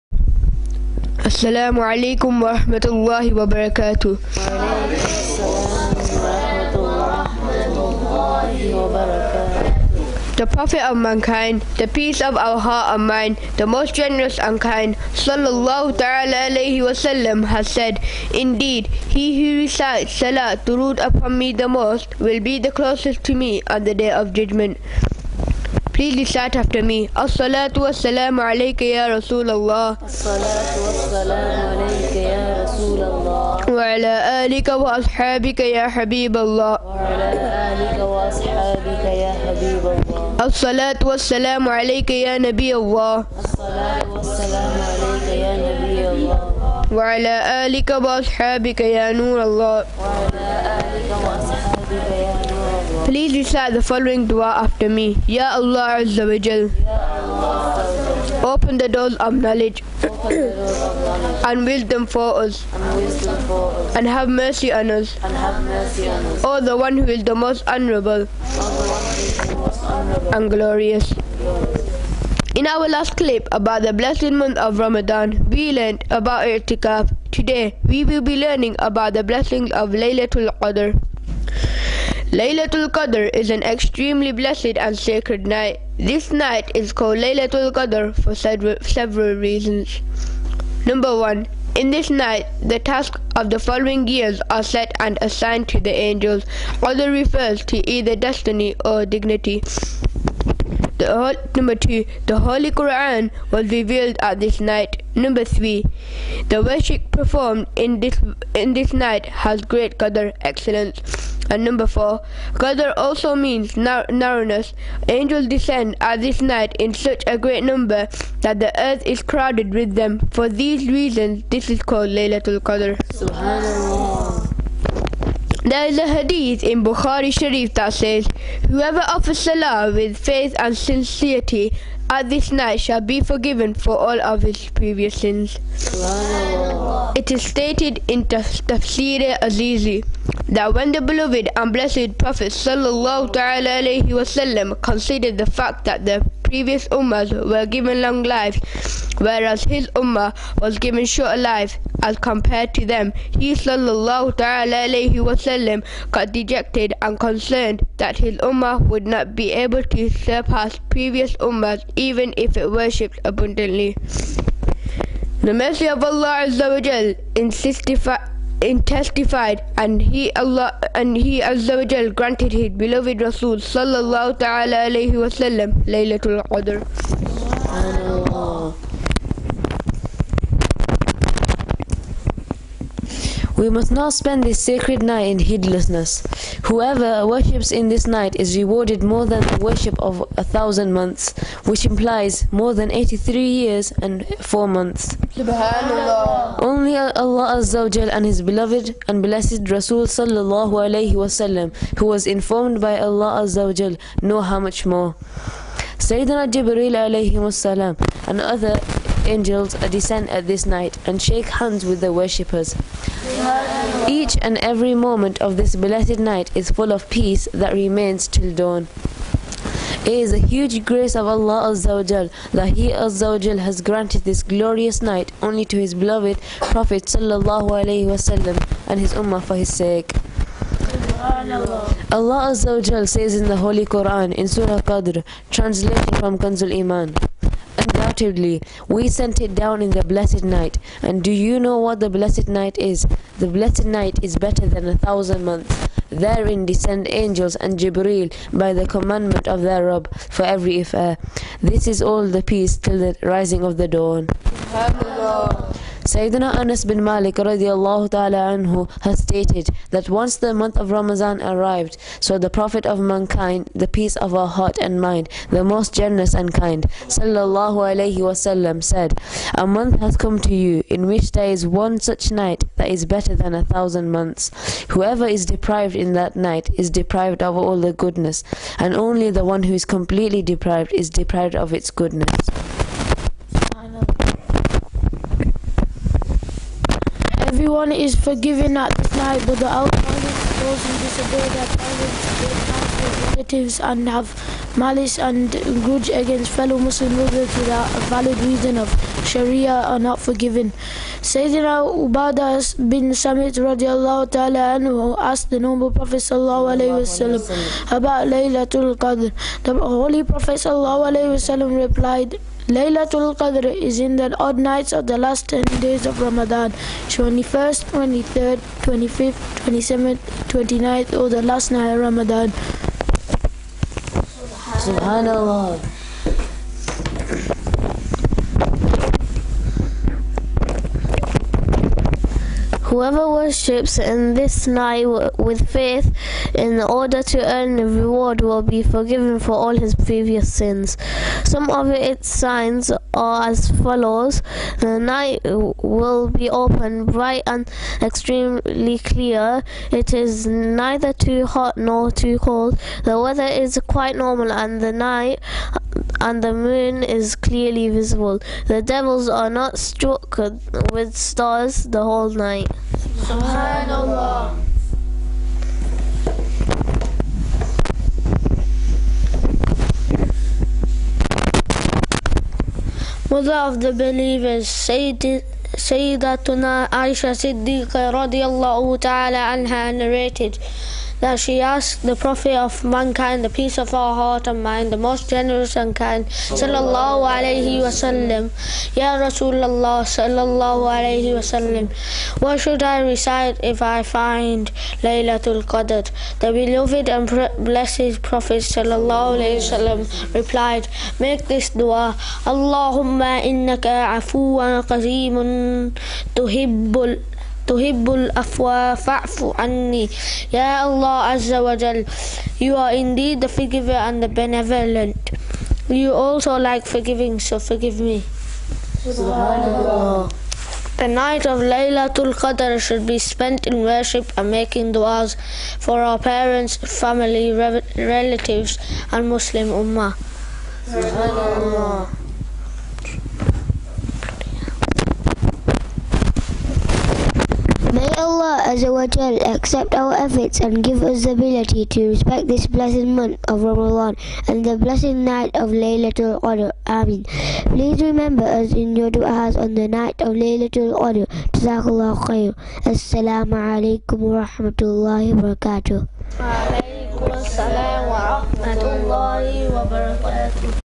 Ramadhan Speech 7 - The Blessings of Laylatul-Qadr
Students of Masjid-E-Aqsa, Preston, explain the blessings of Laylatul-Qadr.

Students: Uzair Iqbal, Momin Farooq, Huzaifah Ali, Rehan & Faraaz Ali
Visitor: Muhammad SafiUllah